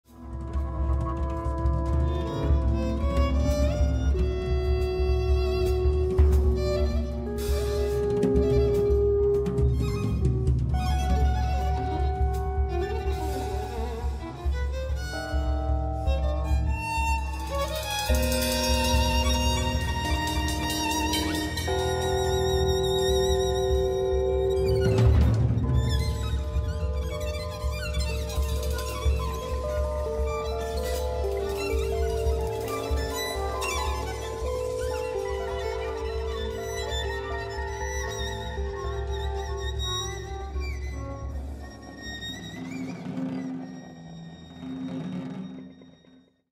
Susan Alcorn Quintet - Jazzfest Berlin 2021 (clip 2)
Susan Alcorn Quintet at Jazzfest Berlin
@ Silent Green

Susan Alcorn - Pedal Steel Guitar
yuniya edi kwon - Violin
Mary Halvorson - Guitar
Michael Formanek - Bass
Ryan Sawyer - Drums